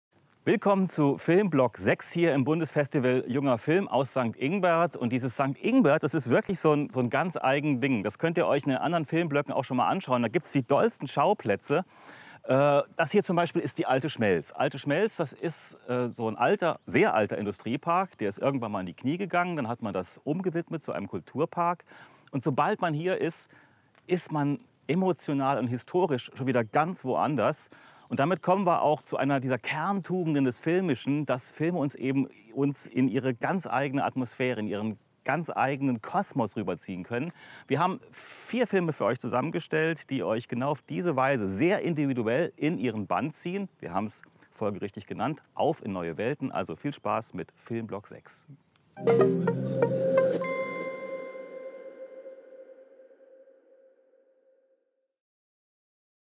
Zum dahinschmelzen: Die Alte Schmelz St. Ingbert - Bundesfestival junger Film 2020
St. Ingbert wird jedes Jahr zur Filmstadt, wenn es heißt: Vorhang auf und Film ab für die spannendsten, lustigsten und dramatischsten Filme des Jahres. Wir begeben uns mit den SR-Moderatoren Oliver Hottong, Isabel Sonnabend und Carl Rolshoven auf Erkundungstour durch die heimliche Kulturhauptstadt.